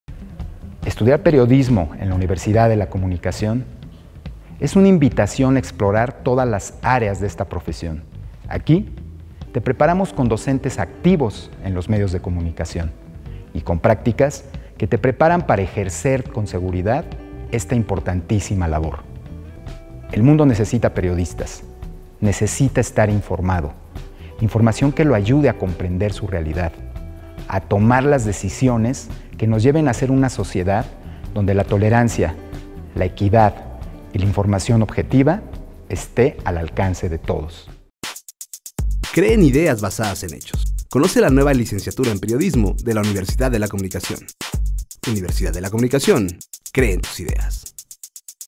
Conoce la licenciatura en Periodismo / Universidad de la Comunicación
Te invitamos a conocer la licenciatura en Periodismo de la Universidad de la Comunicación
¡Inscripciones abiertas!